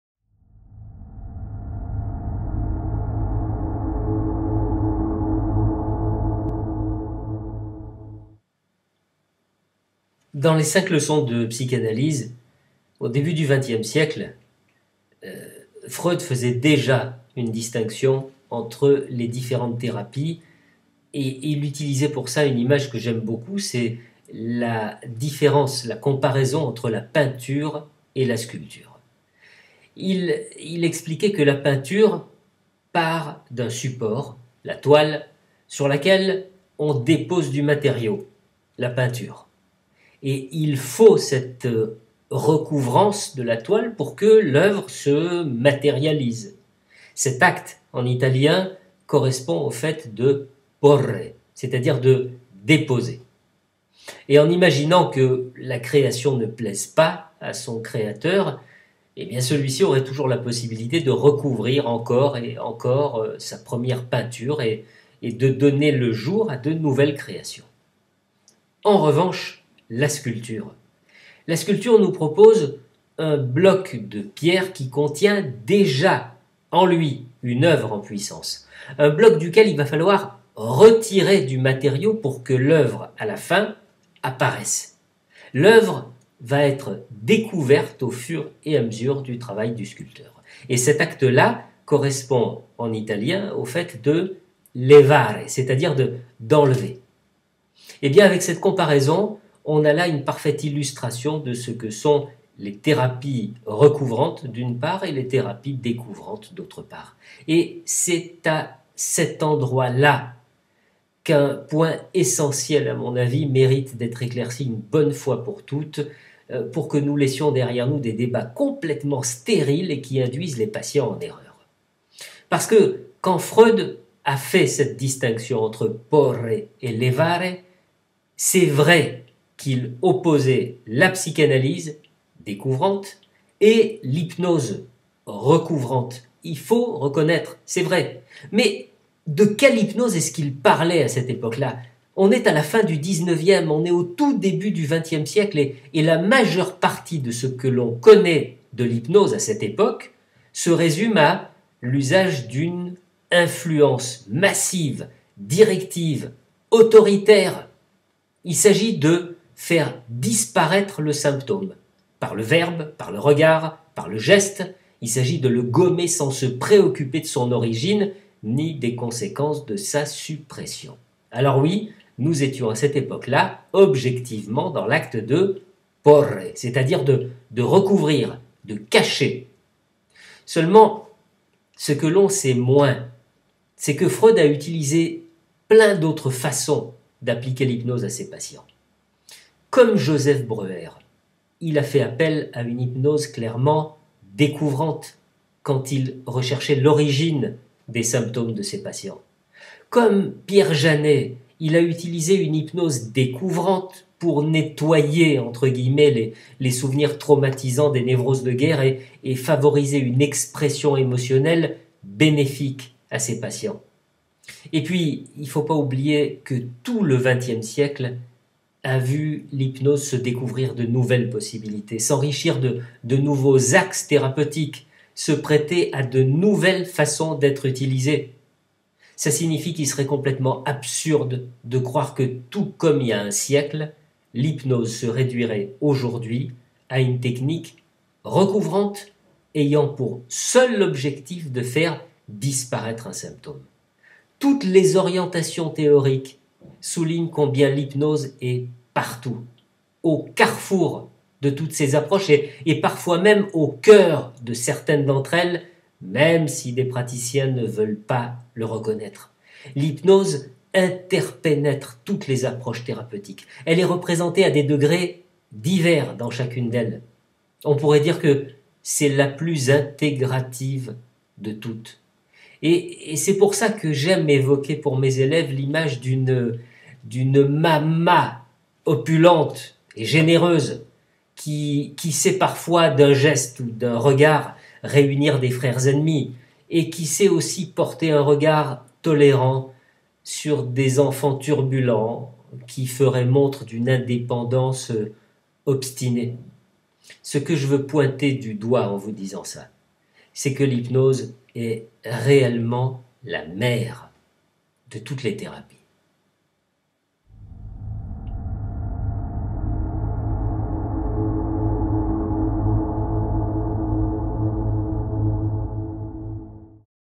Porre o levare
Peinture ou sculpture ? C'est en comparant ces deux arts que FREUD faisait déjà il y a plus d'un siècle la différence entre la psychanalyse et l'hypnose. Mais quelle hypnose ?...